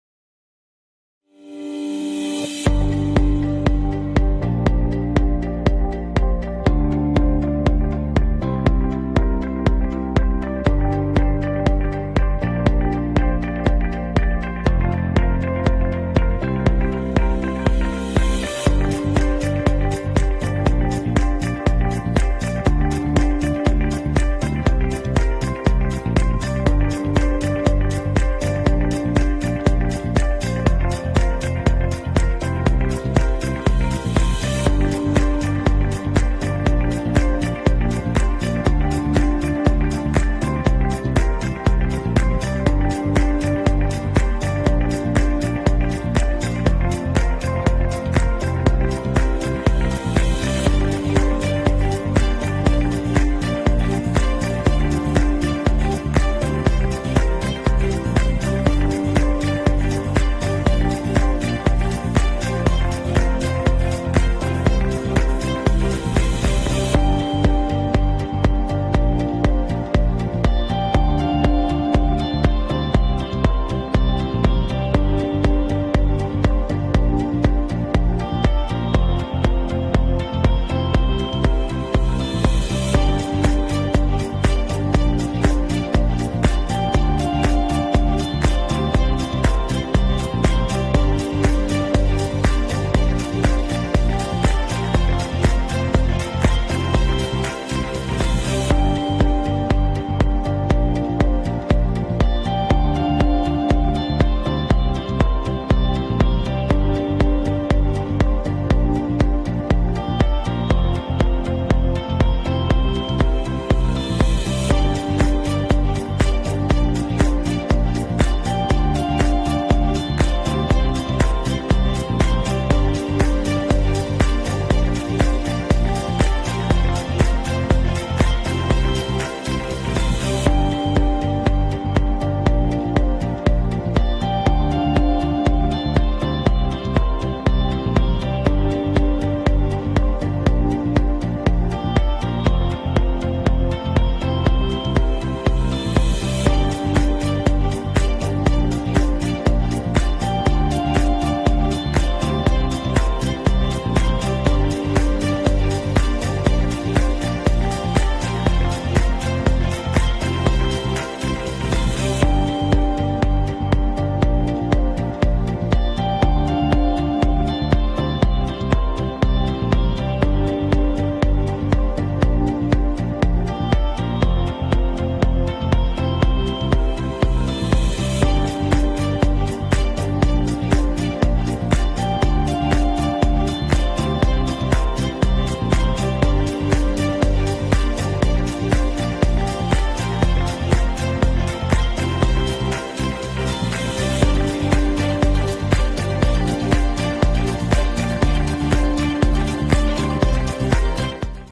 HEATED TOWEL RAIL - SMART THERMOSTAT - HOW DOES IT WORK?
In the video we can see how the smart thermostat works step by step. In addition, we can also see all the differents modes or programs that the thermostat can offer: Comfort Mode, Antifreeze Mode, Boost Mode, Timer 24 Mode and Timer 12 Mode.